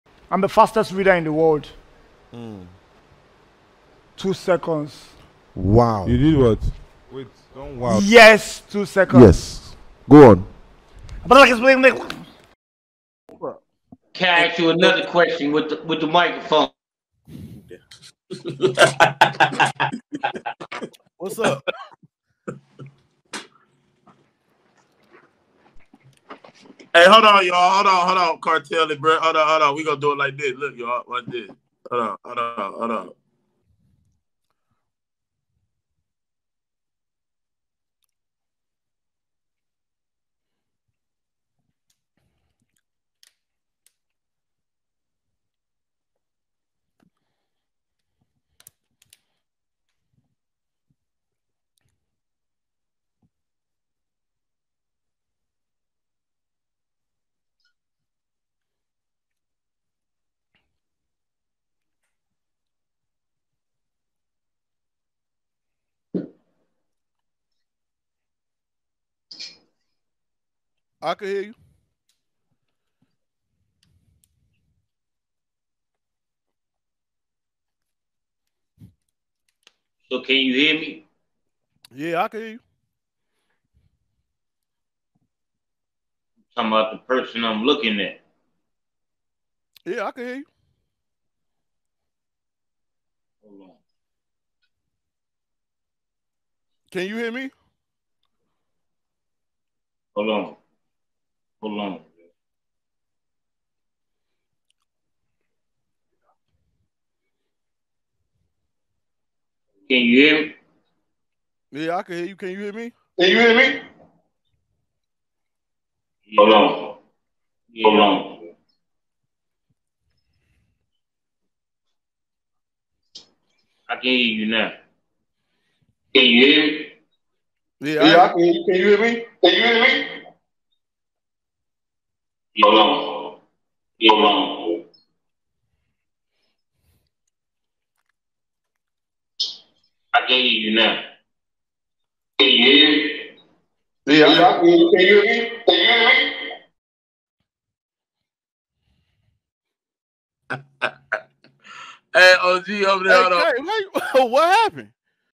King AK47 Funny Live, Can you hear me?
Before the video stats OG has already asked can you here me a dozen times comment section is dying